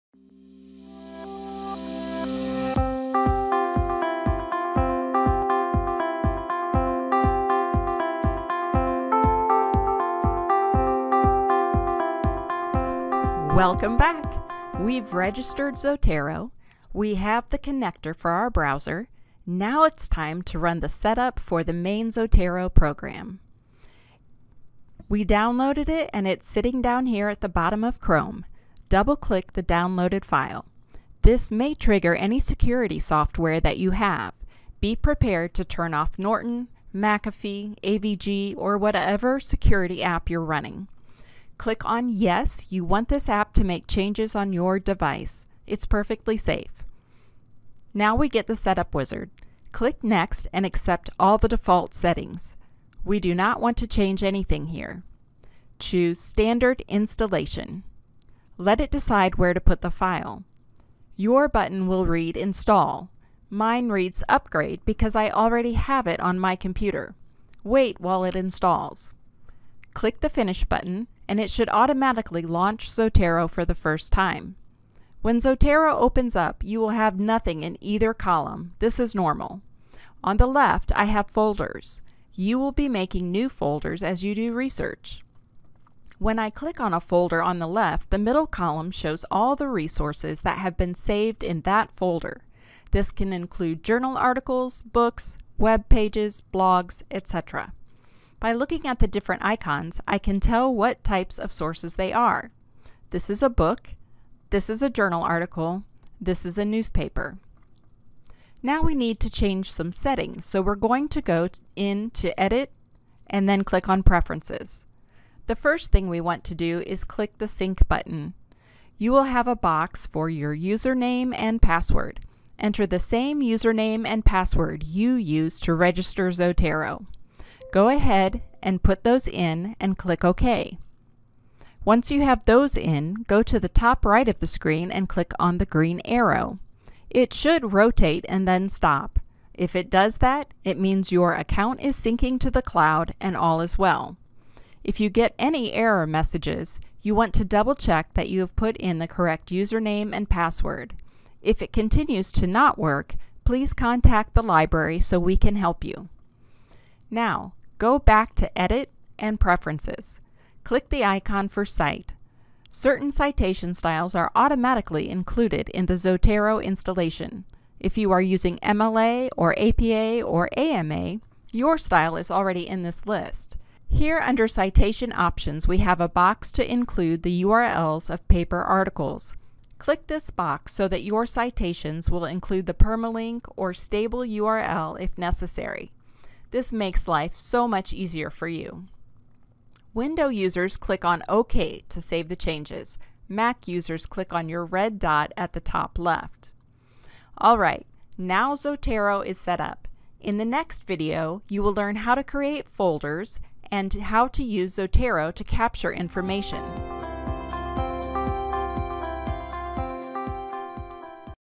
Zotero setup
How to change settings in Zotero to get the best user experience.